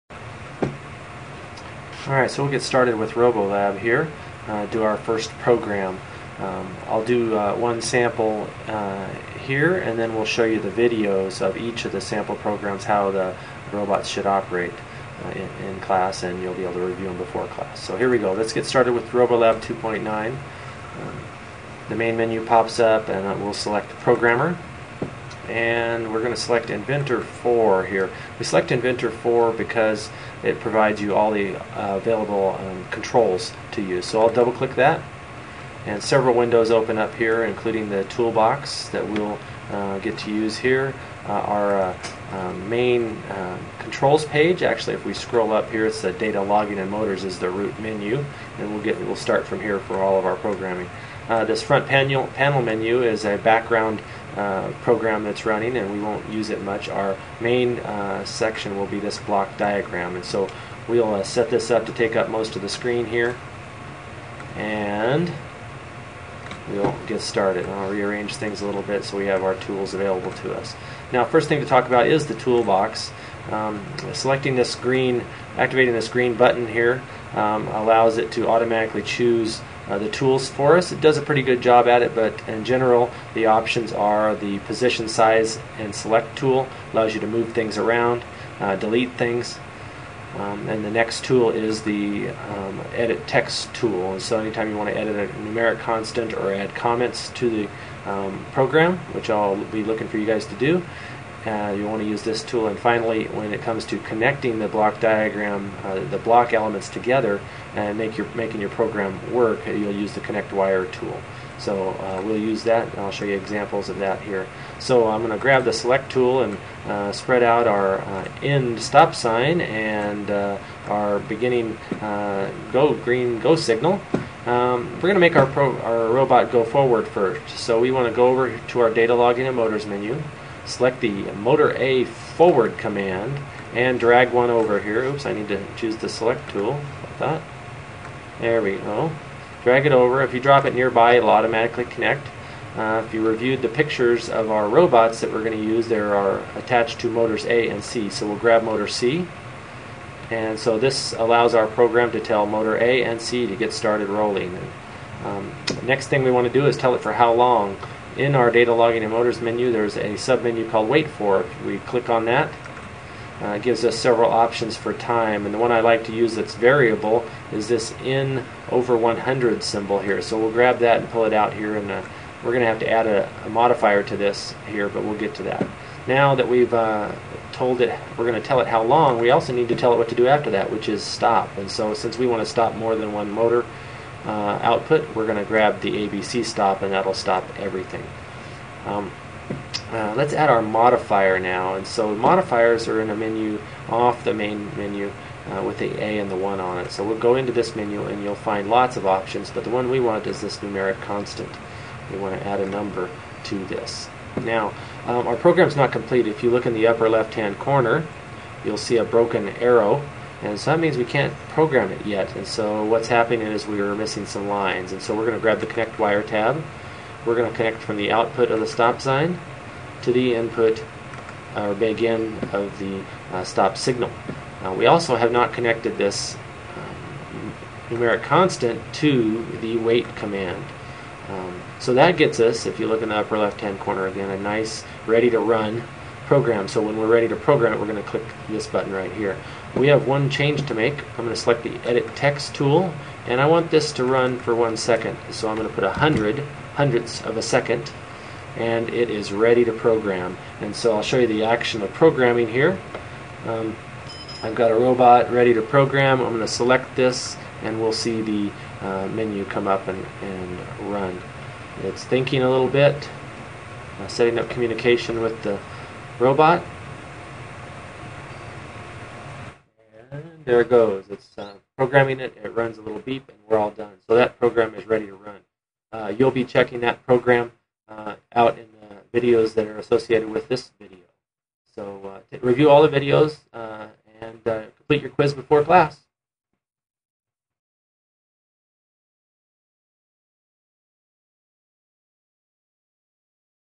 Robot Program 1-ENGR 120
Getting started with programming the Lego RCX controller for ENGR 120. How to open and program using the RoboLab 2.9 environment. Make sure and watch the expected robot performance for each of the sample programs to be completed during Week 1 of our programming activities.

Timed Motion...Quiz Question: "If you don't see the Functions or Tools Palettes, which pull-down menu do you use to restore them?"
ANSWER: Windows...hit this pull-down menu and select the Palette you are missing. This usually happens when you close one by accident.